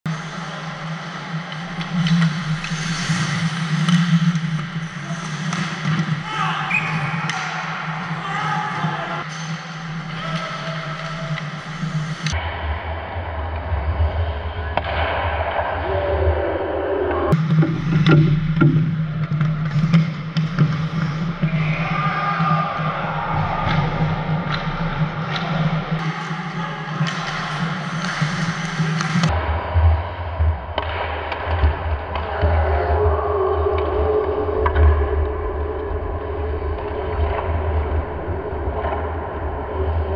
Nice little 2nd period for the boys
down 3-0 going into the 2nd the boys came out firing on all cylinders